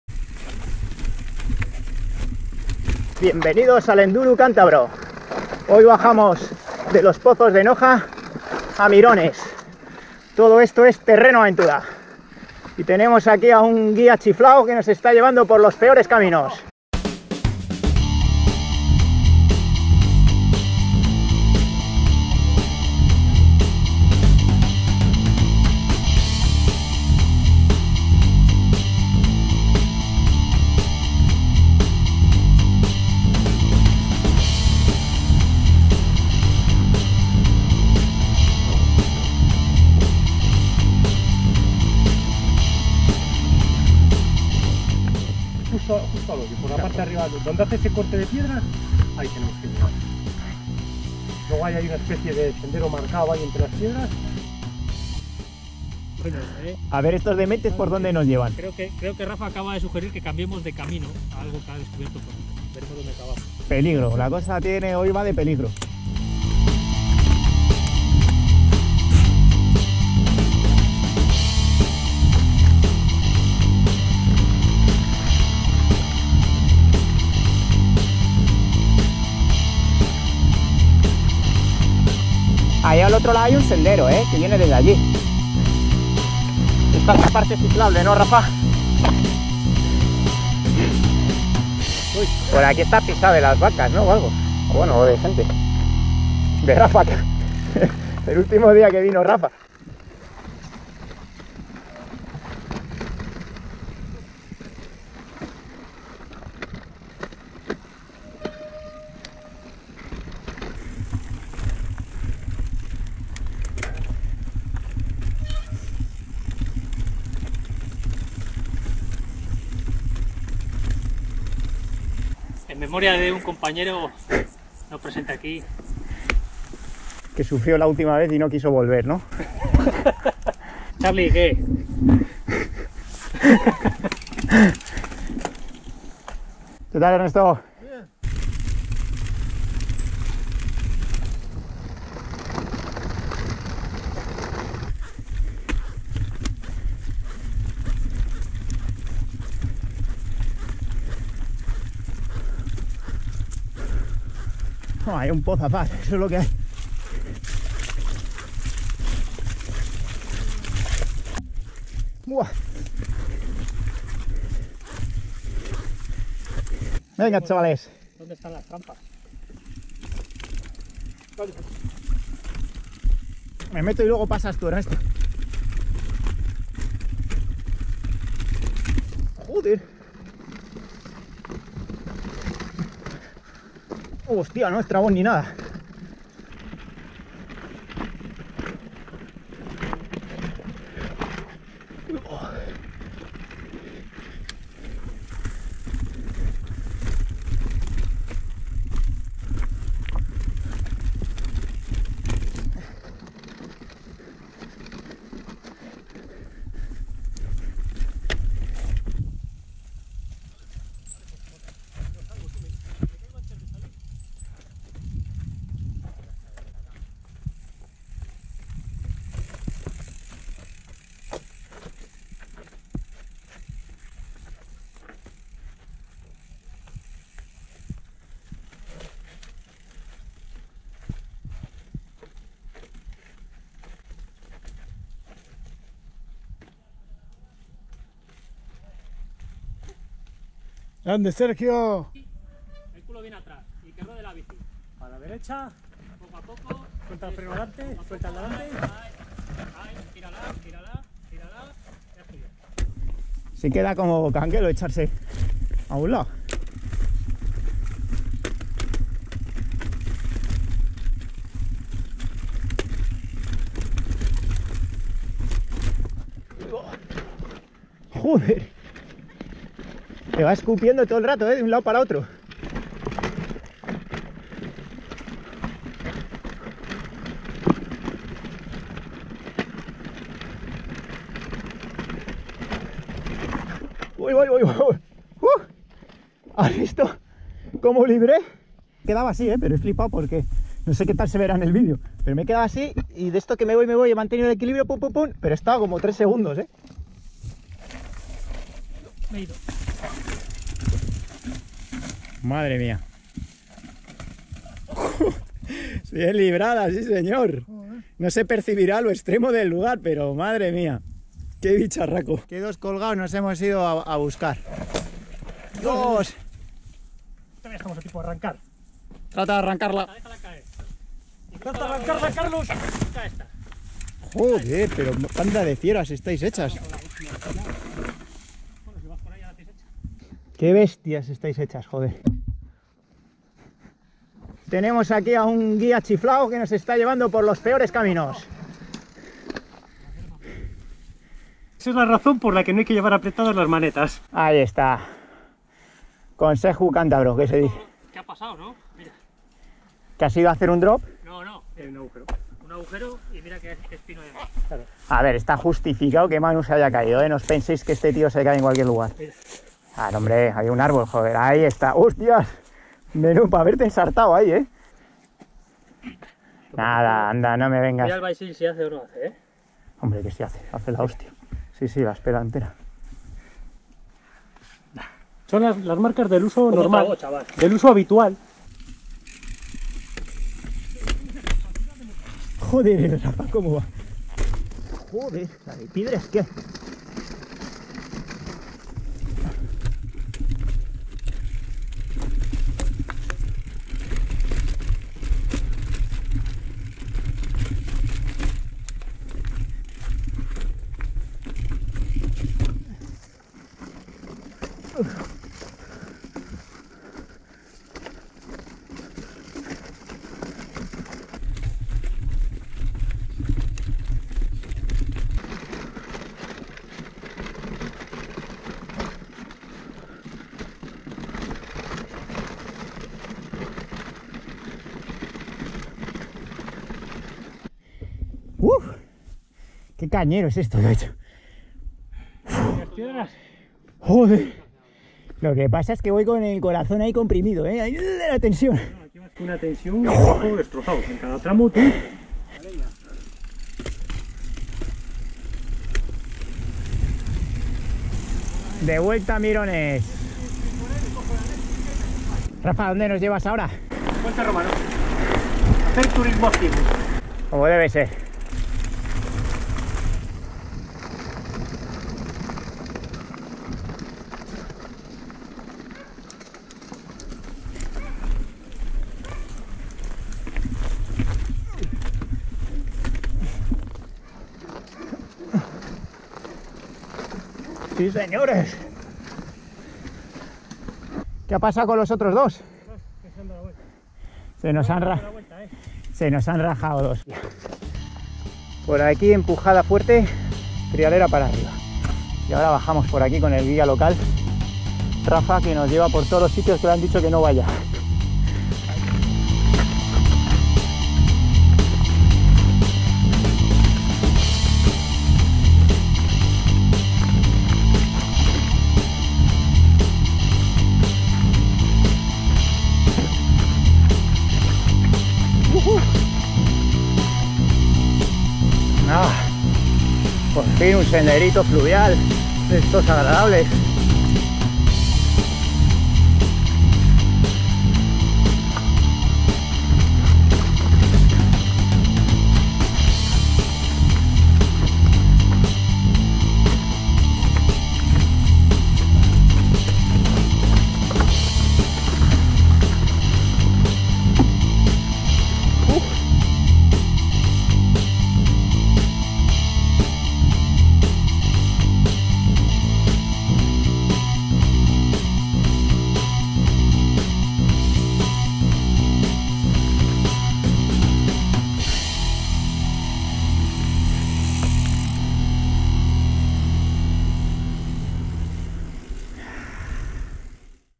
Bajada Salvaje de los Pozos Noja a Mirones
Buscamos suscriptores. ¿Te animas?

Si te ha gustado este video, no lo dudes: dale al “me gusta” y comparte.

Ruta no apta para cardiacos… La “Bajada Salvaje” discurre por una antigua cambera de losas (descolocadas) de piedra que arranca en las inmediaciones de los Pozos de Noja y termina en el fondo del valle del río Miera, en el pueblo de Mirones (Cantabria).

Aunque físicamente no es una ruta especialmente dura, es un auténtico reto técnico. La subida es cómoda hasta los Pozos de Noja (por carretera comarcal y el último trecho por pista). Ahí comienza la aventura para acceder al inicio de la “Bajada Salvaje”, transitando por brañas y esquivando algún resalte rocoso que requiere cortos porteos. El sendero de bajada es una ensalada de piedras de todos los tamaños, en ocasiones discurre junto a un barranco con cierta exposición y hay más de un tramo de trialeras con fuerte pendiente. Si además lo pillas mojado, la cosa se complica…

Paisajes espectaculares del valle de Miera, aventura, adrenalina y buena compañía de los máquinas de Enduru Emetebe. ¡Gracias a Rafa por guiarnos en esta ruta de enduro-montañismo!